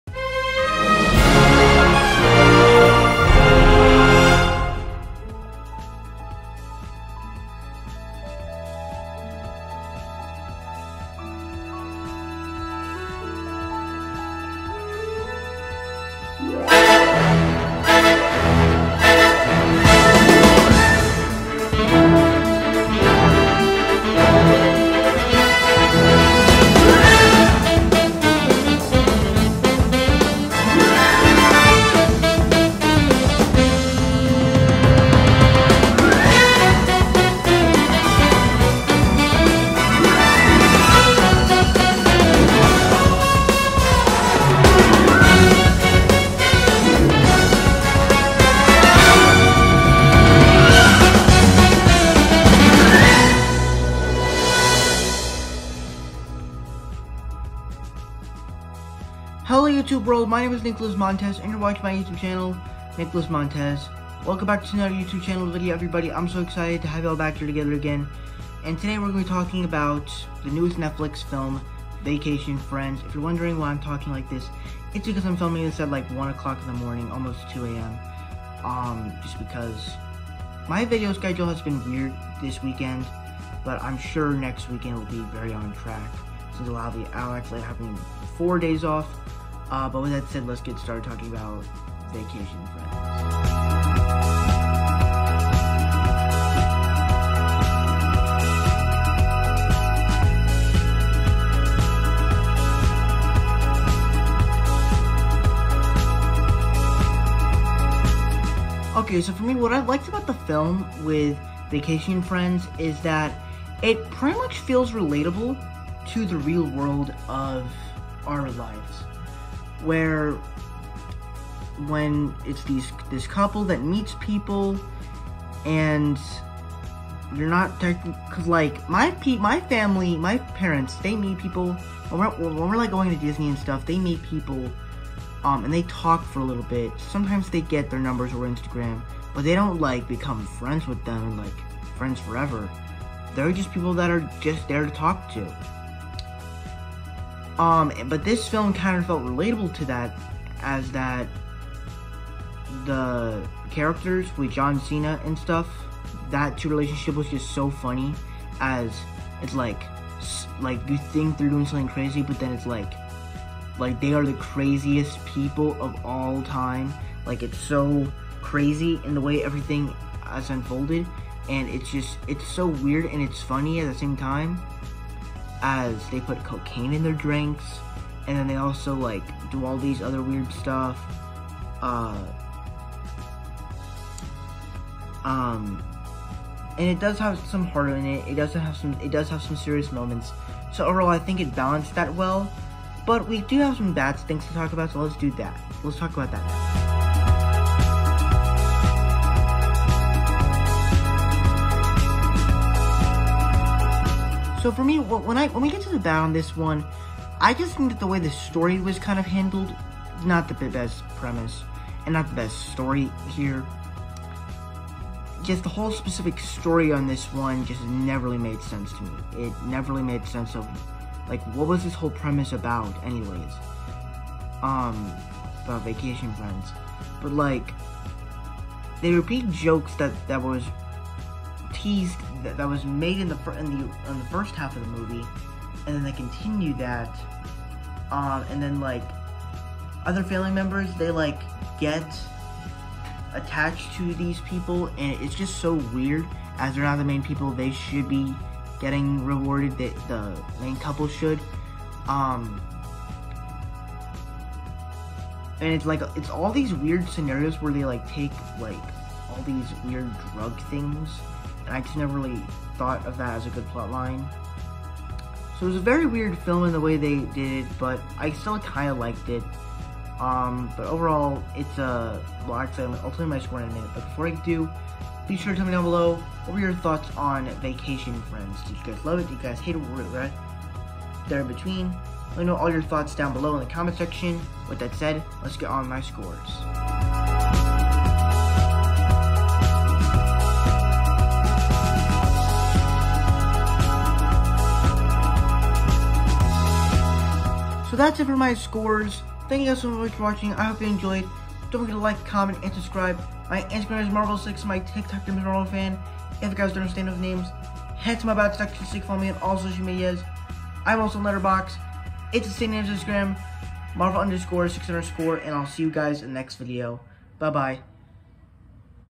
Vacation Friends (2021) - Netflix Movie Review
Content Chapters:
00:00 Intro
01:35 The Good
03:20 The Bad
04:48 Scores and Closing